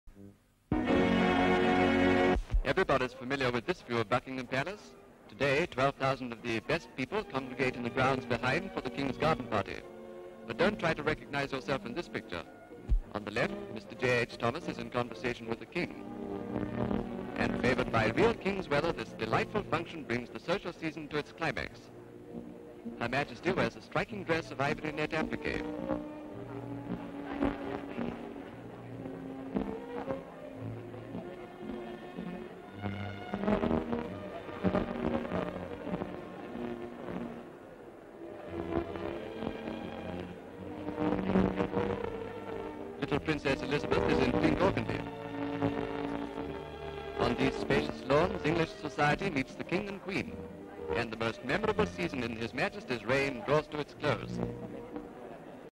UNITED KINGDOM: King George V holds Royal Garden Party in the grounds of Buckingham Palace (1935)
King holds George V holds Royal Garden Party in Buckinham Palace grounds. Twelve thousand guests?

Full Description:

SLATE INFORMATION: THE KING'S 12,000 GUESTS Royal Garden Party climaxes Season. Brilliant function in Buckingham Palace grounds.

ENGLAND: London

EXT
Gen. view of Buckingham Palace. Gen. view pan. of Guests in Garden. Semi view King George V & Queen Mary; King talks to J.H. Thomas. CU. of Queen Mary talking to man. Top view of King shaking hands with men. CU. of King talking to 2 men. Top view of Guests. CU. of Duchess of York & Princess Elizabeth being kissed. Top view of Guests. Semi cu. of King and Queen shaking hands with Guests.

Sound Effects: Off-stage voice describes Royal Garden Party
Background: King holds George V holds Royal Garden Party in Buckinham Palace grounds. Twelve thousand guests attend.